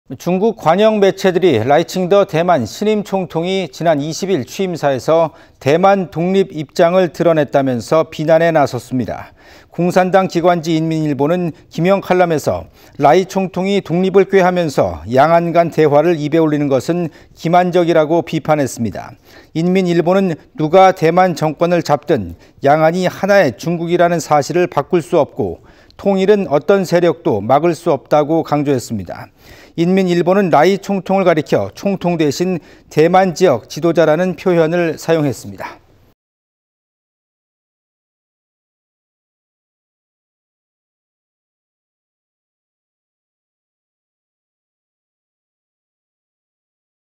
중국 관영매체, "대만 신임 총통이... 대만 독립 입장 밝혀" 비판 일침
중국 관영 매체들이 라이칭더 대만 신임 총통이 지난 20일 취임사에서 '대만 독립' 입장을 드러냈다면서 비난에 나섰습니다.

공산당 기관지 인민일보는 기명칼럼에서 라이 총통이 독립을 꾀하면서 양안 간 대화를 입에 올리는 것은 기만적이라고 비판했습니다.

인민일보는 누가 대만 정권을 잡든 양안이 하나의 중국이라는 사실을 바꿀 수 없고, 통일은 어떤 세력도 막을 수 없고 강조했습니다.

인민일보는 라이 총통을 가리켜 '총통' 대신 '대만 지역 지도자'라는 표현을 사용했습니다.